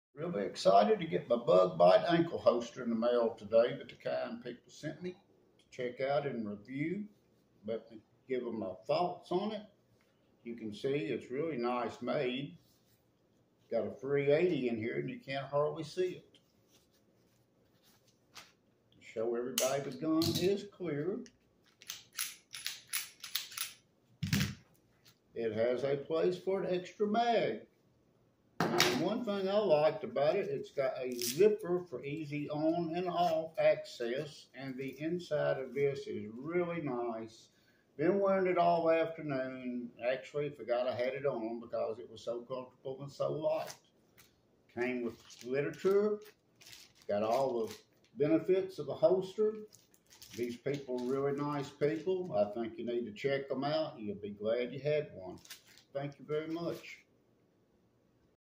Bitten by BugBite Ankle Holster
Received this BugBite Ankle Holster today to review. Very comfortable, well made and concealable. Been wearing all afternoon and could not really tell it was on.
 Many features that I liked. With the zipper it aids in taking on and off. The firearm stays secure and the pouch for extra mag has a Velcro strap and a strap at the top to aid with the tension. It exceeds the pictures when you have it in hand.